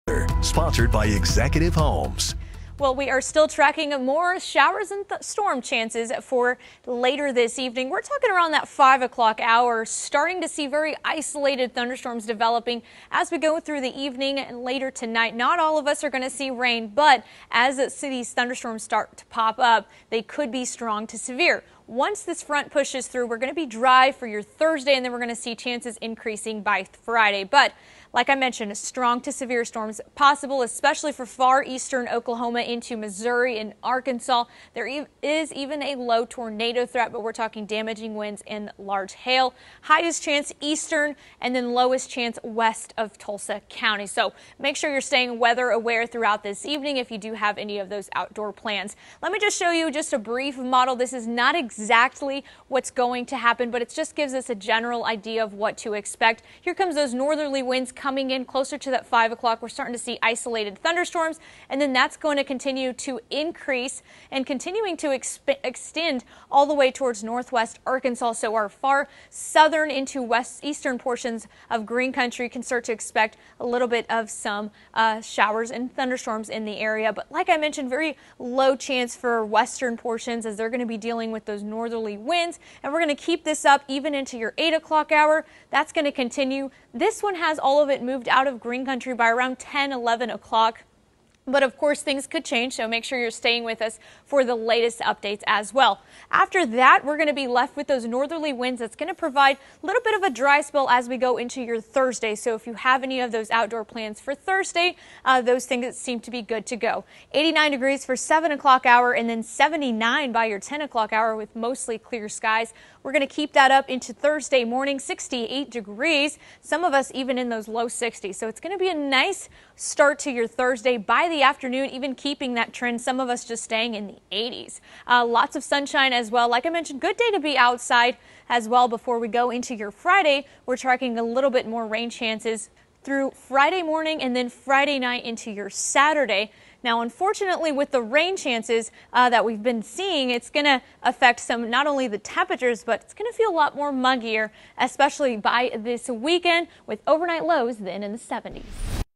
More Storm Chances Tonight
Meteorologist Emma Landeros has your forecast for Aug. 9, 2023.